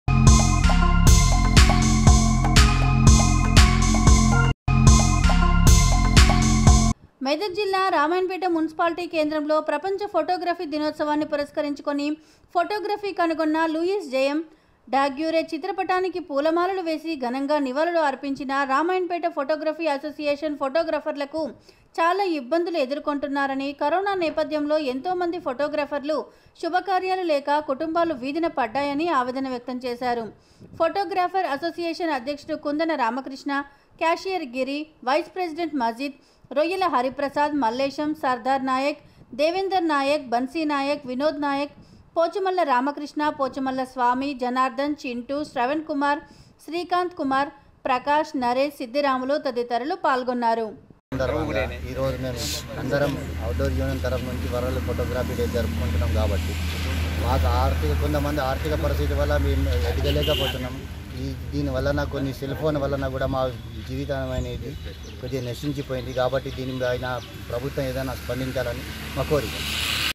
రామాయంపేటలో ప్రపంచ ఫోటోగ్రఫీ దినోత్సవం
రామాయంపేట మున్సిపాలిటీ కేంద్రంలో ప్రపంచ ఫోటోగ్రఫీ దినోత్సవాన్ని పురస్కరించుకొని. ఫోటోగ్రఫీ కనుగొన్న  లూయిస్ జేయం. డా గ్యురే  చిత్రపటానికి పూలమాలలు వేసి  ఘనంగా నివాళులు అర్పించిన రామయంపేట  ఫోటోగ్రఫీ  అసోసియేషన్